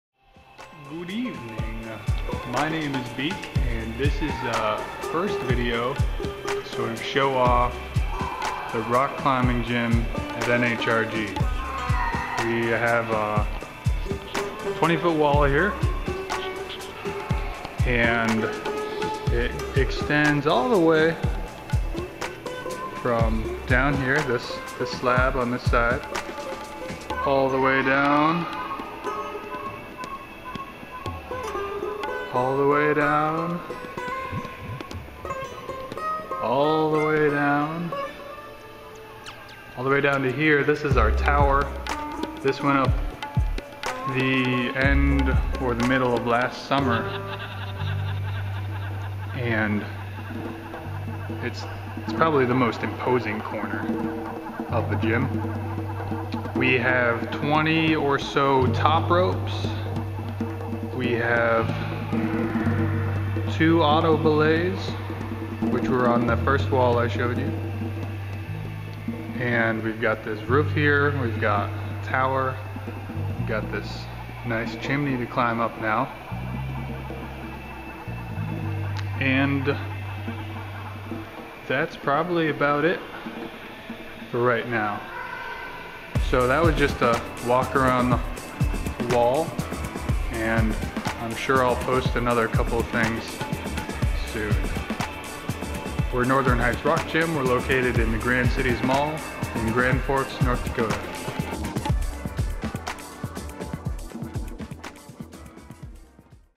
Northern Heights Rock Gym Shorts - Episode 1 - Introduction
Just a quick walk around the climbing wall describing some of what we have.  Narrated by Matt BK.